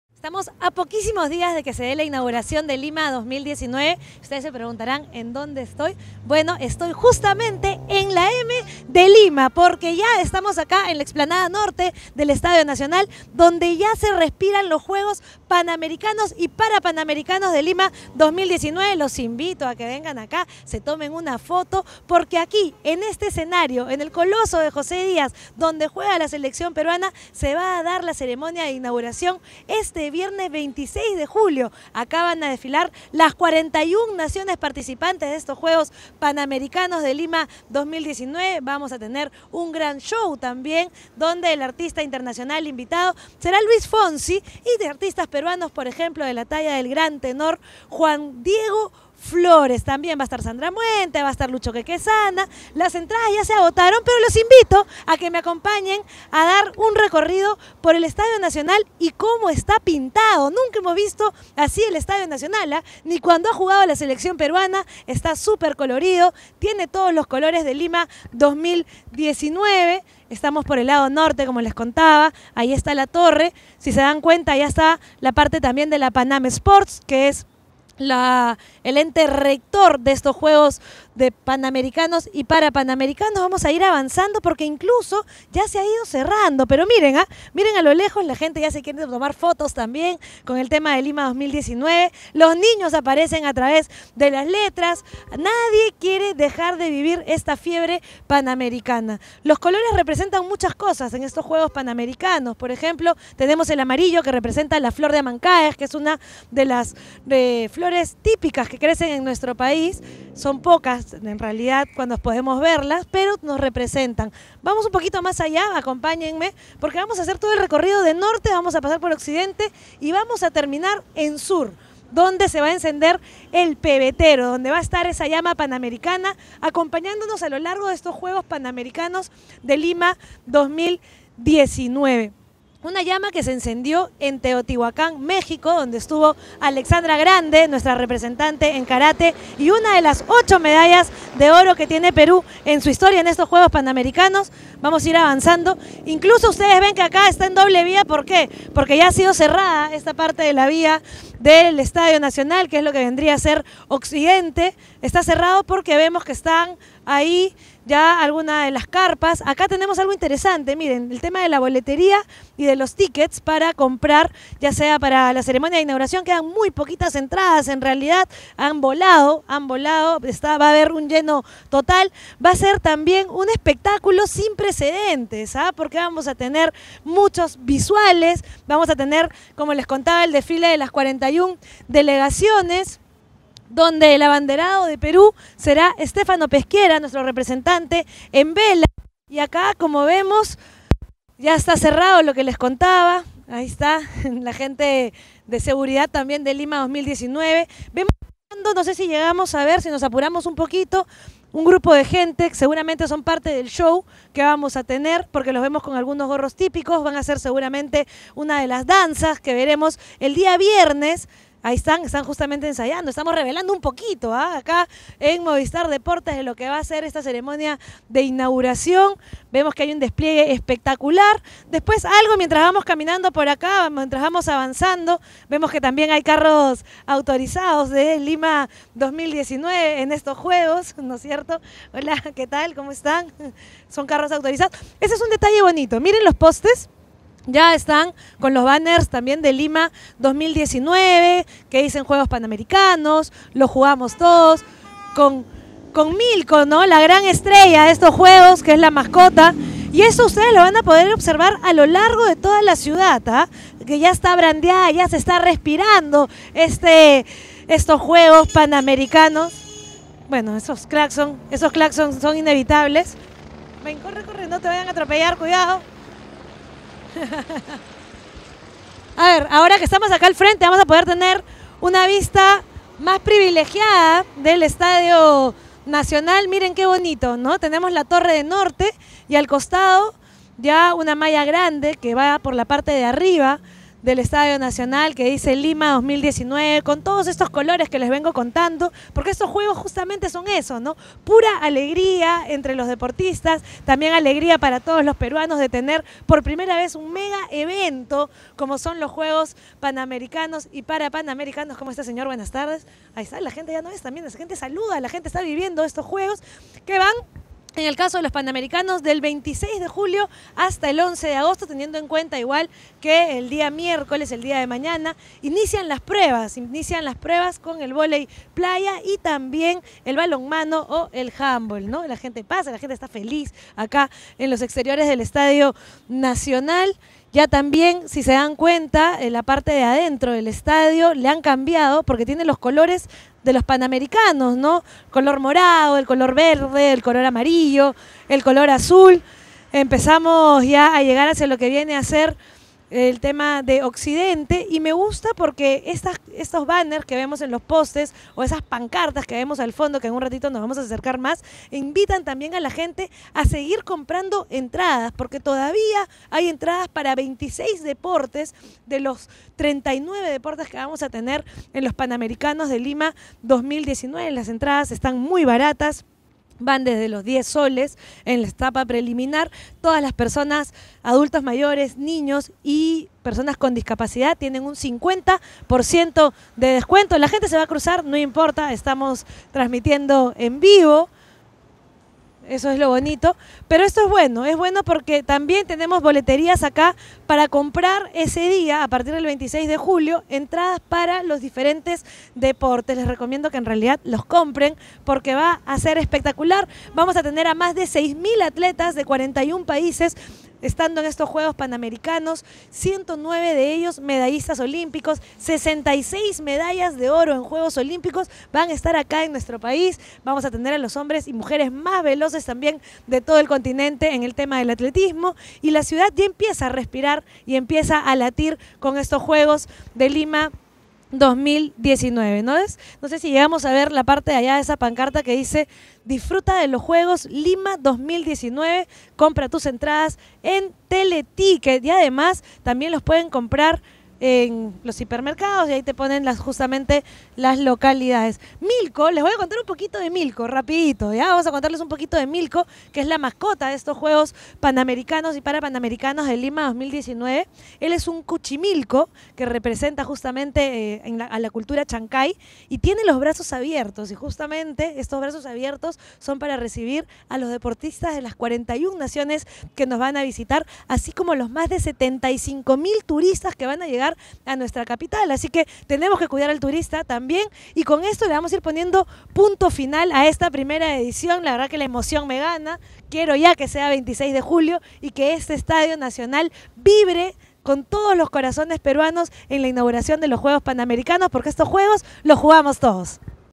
ElVlogdeDaniella: a tres días de los Juegos Panamericanos Lima2019 | CAP #1
¡FALTAN HORAS PARA Lima2019! Por ello, Daniella Fernández nos cuenta sus expectativas del certamen deportivo que paralizará nuestra ciudad. ¿Dónde se encuentra? A las afueras del mítico Estadio Nacional🏟.